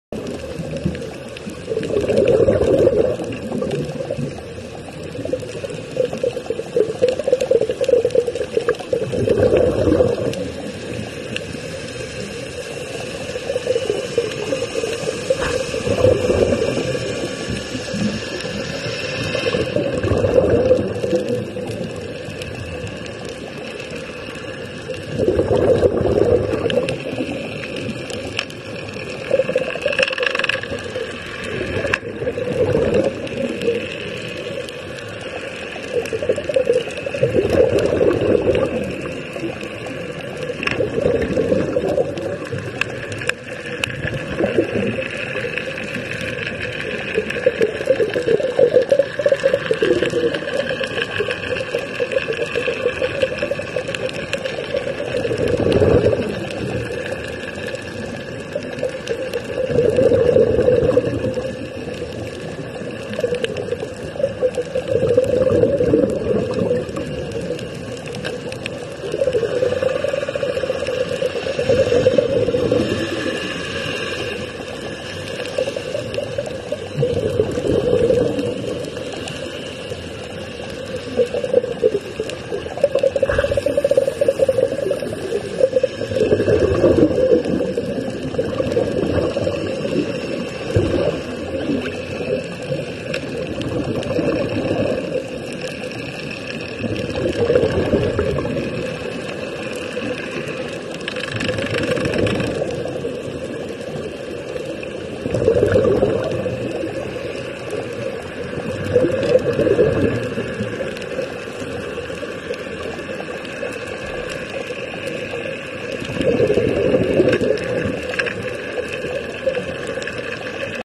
How to Give foods for Fishes Under the Sea
In the Sri Lanka sea side someone gives food for the fishes under the sea.